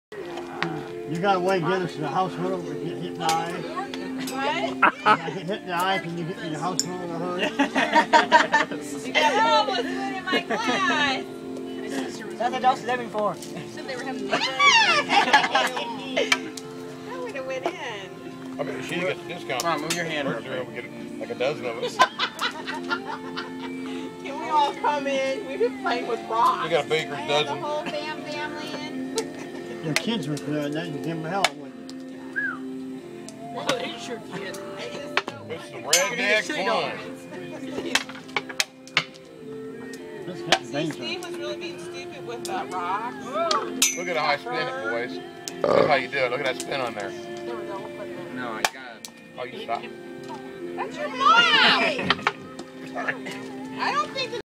Hillbilly Camping game
Sit in a circle and throw rocks at a half full can of beer hanging from a string